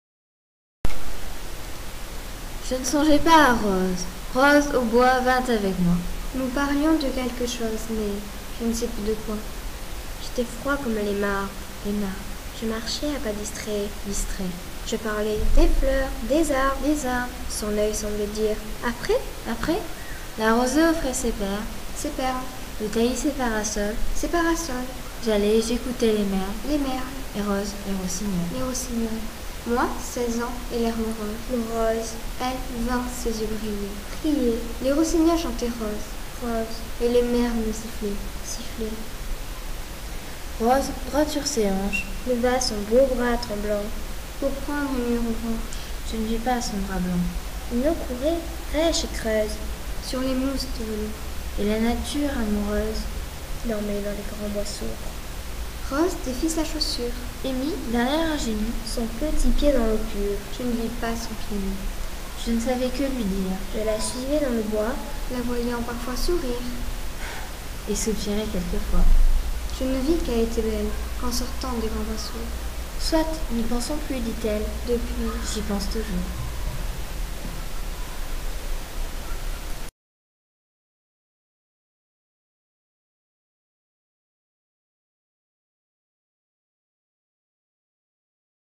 Vieille chanson du jeune temps de Victor Hugo
Vieille chanson du jeune temps de Victor Hugo, dire, ensemble, trois voix, cinquième, images exposition BNF "Victor Hugo, l'homme océan", poésie